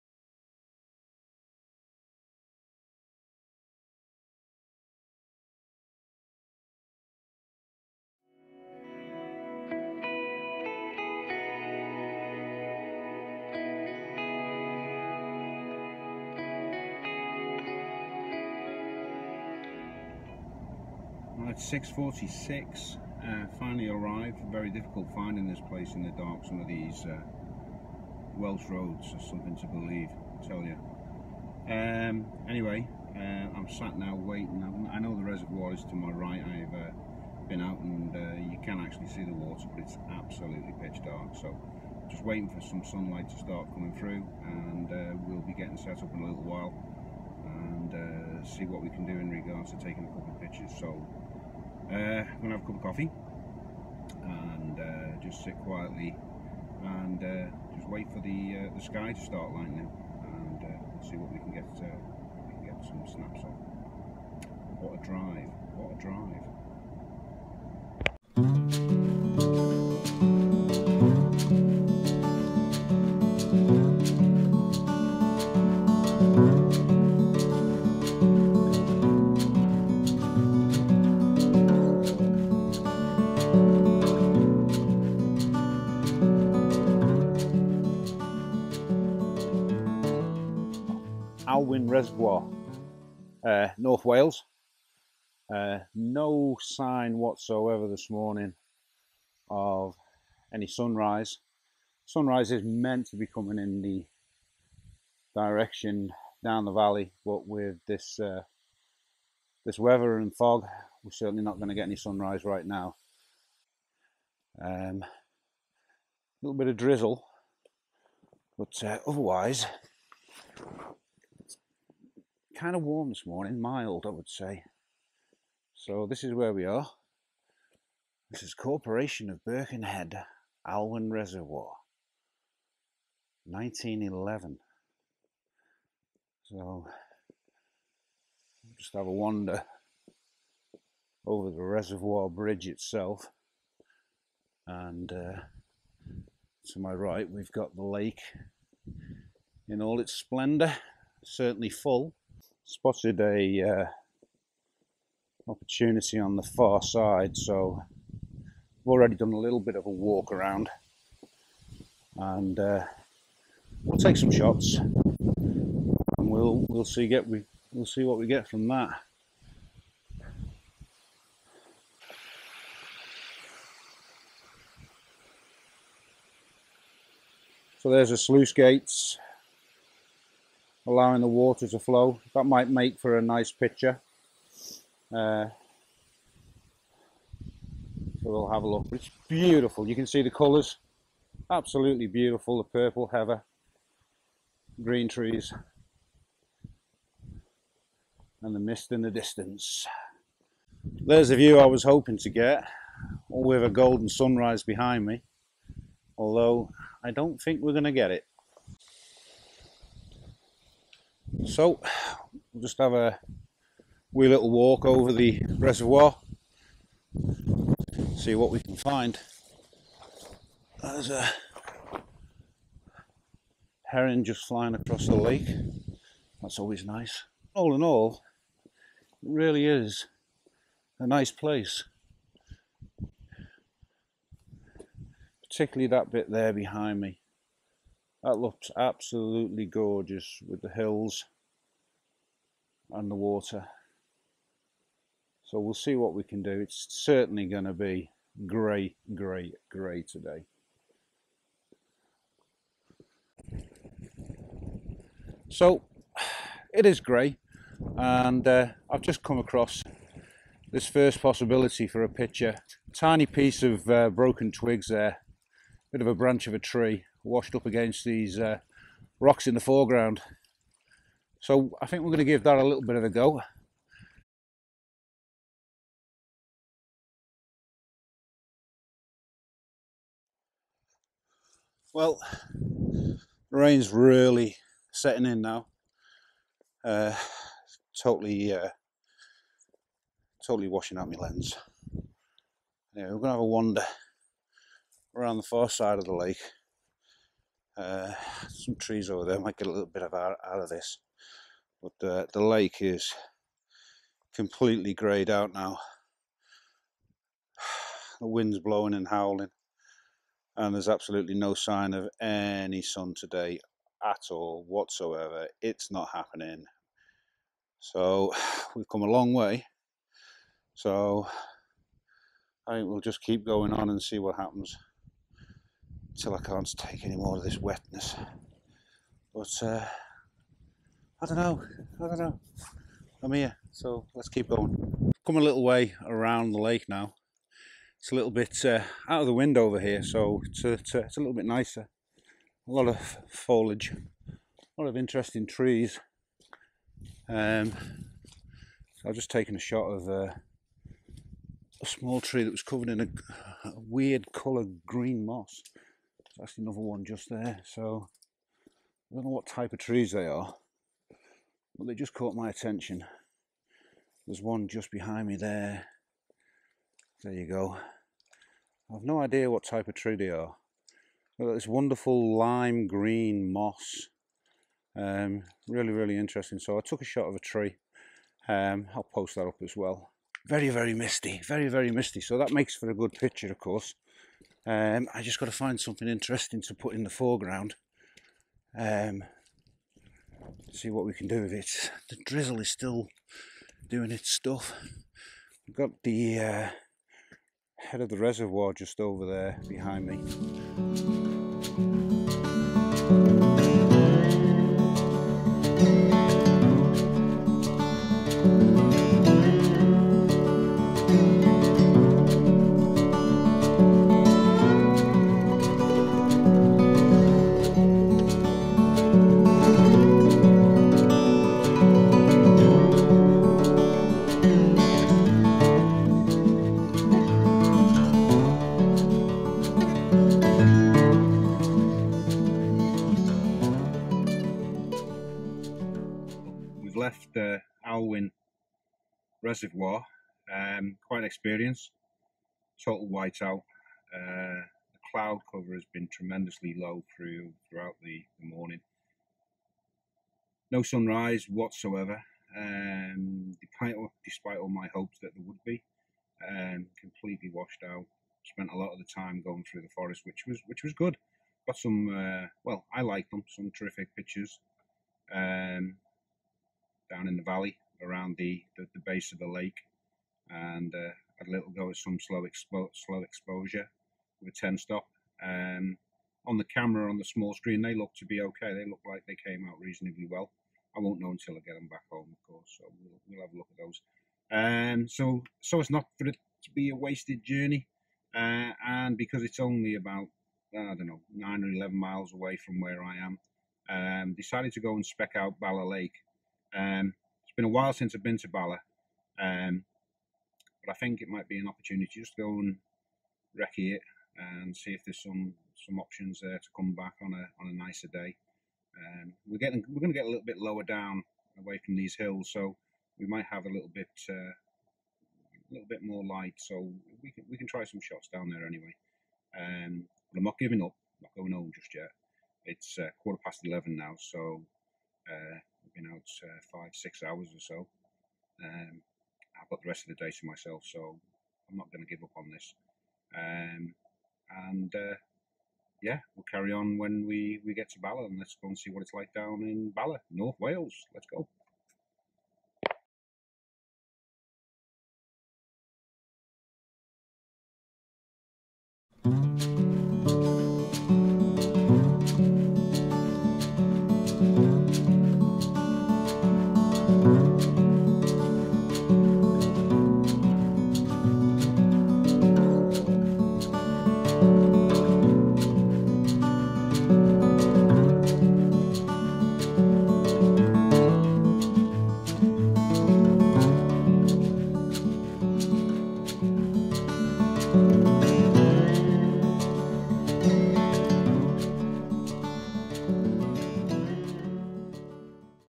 Landscape Photography Alwem Reservoir & Lake Bala North Wales
Back in late November 2017 on a wet cold grey day, took a run out to Alwem Reservoir & Lake Bala North Wales, arrived a full hour before the sunrise that never materialised, but kept going and got a few pictures that I was happy with, enjoyed a nice long walk about one third of the way around the lake with a plan to revisit when weather promises to be better hope you enjoy like and subscribe Thanks for watching Terry